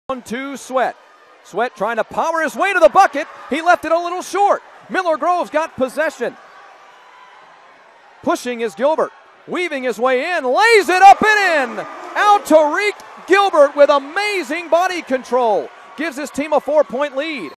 Miller Grove #3 Alterique Gilbert coast to coast layup
2014 Region 6-AAAAA Championship game between Miller Grove and Stephenson.